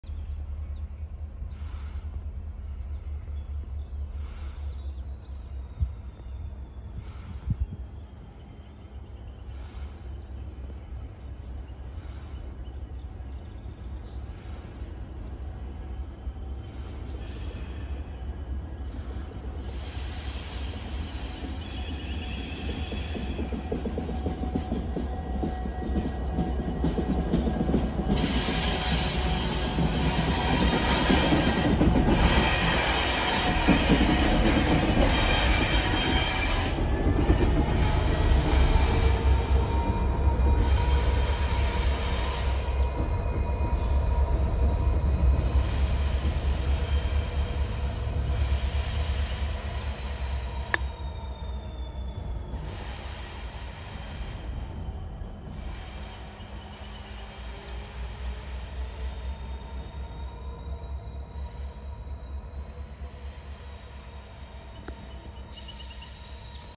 Norfolk Southern H96 Light Engine Move - 5/24/23
A Norfolk Southern H96 light engine move is seen crossing over the Northern Extension - York County Heritage Rail Trail on the York Secondary in York, Pennsylvania at 10:13 AM on Wednesday, May 24, 2023. The engines in this video are: NS 6977 (SD60E). Enjoy!